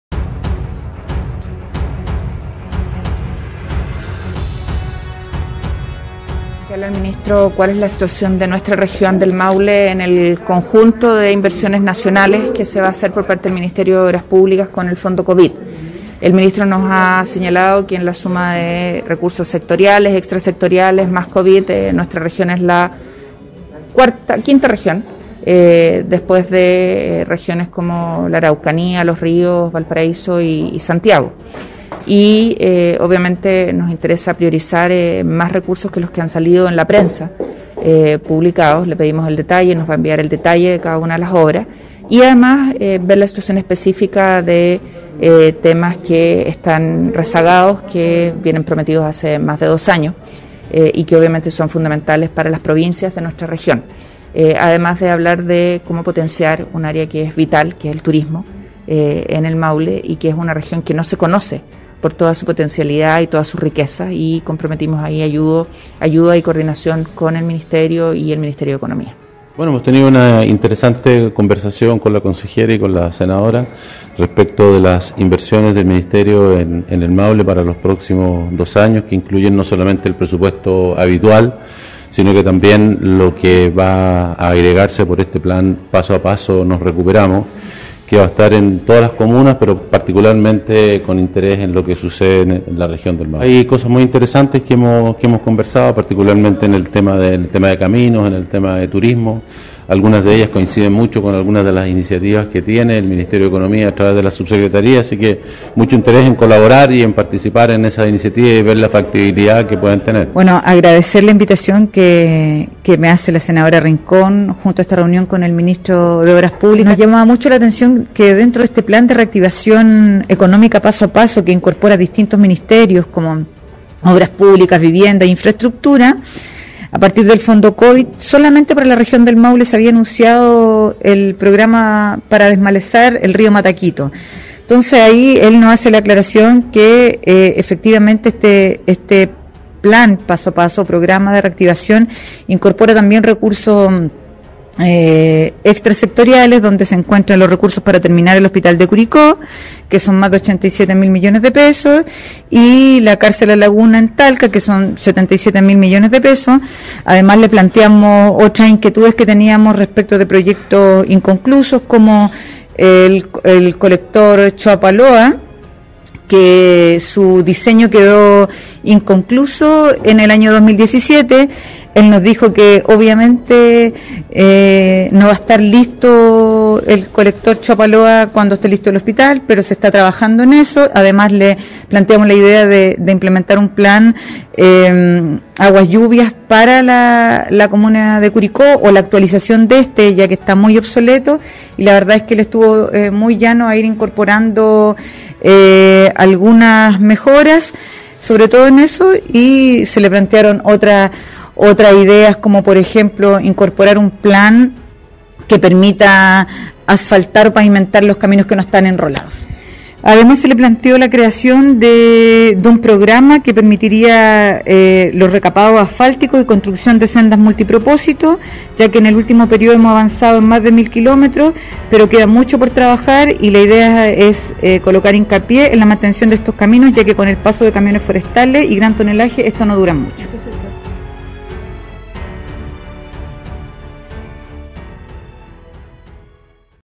Plan “Paso a Paso Chile se Recupera”: de 94 licitaciones solo una corresponde al Maule
Propuestas. Ante tal escenario, la senadora Ximena Rincón y la consejera regional Cristina Bravo, se reunieron con el Ministro de Obras Públicas para plantear su preocupación.